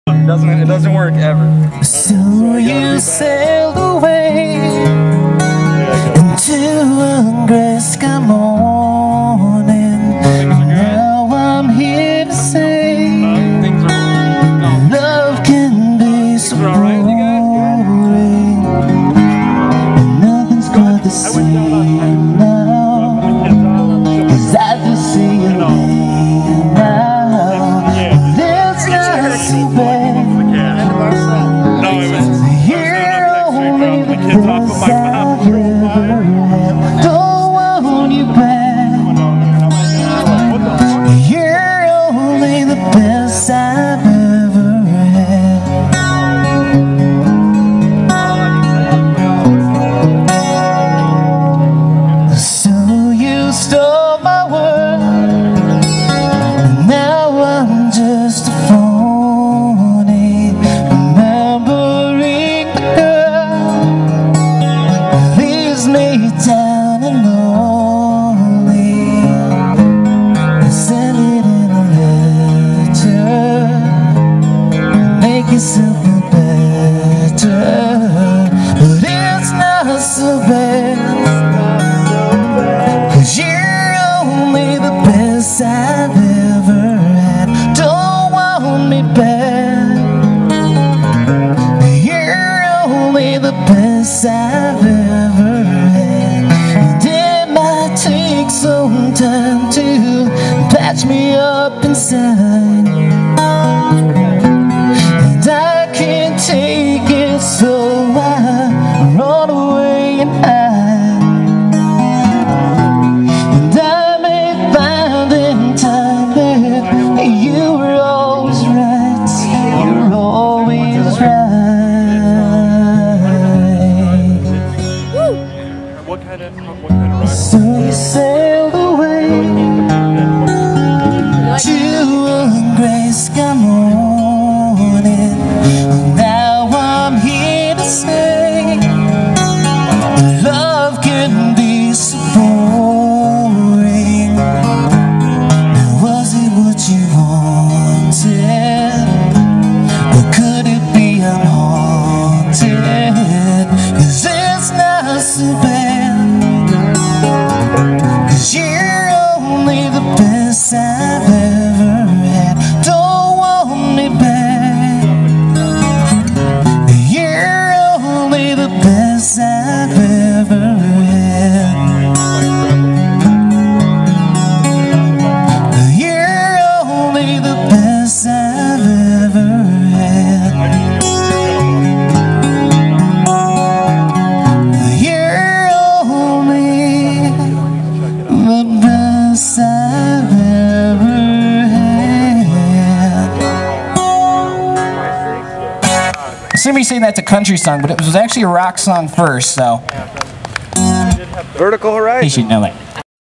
Rockstock Justino 2
Justin, Justino playing a Vertical Horizon cover at Muskegon Rockstock 08.  6/21/08 Hackley Park.